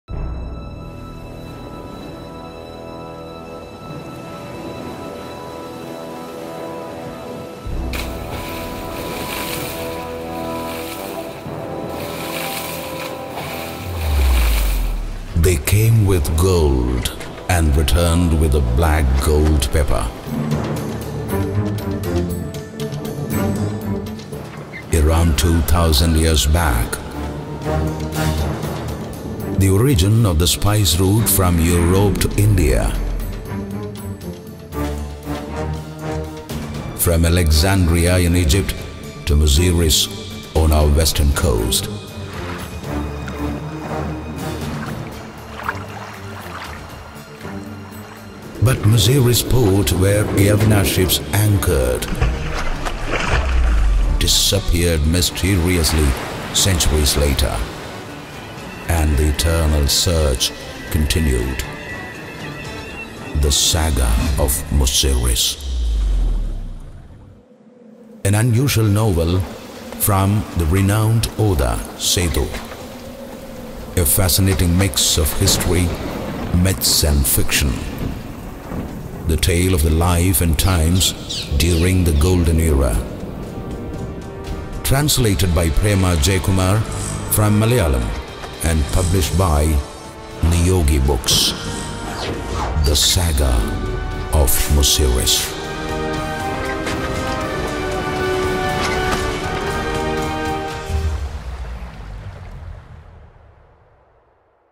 THE SAGA OF MUZIRIS : BOOK TRAILER 01
A PATH BREAKING NOVEL BY SETHU
TRANSLATED BY : PREMA JAYAKUMAR
TRAILER BY : ANILPUNNAD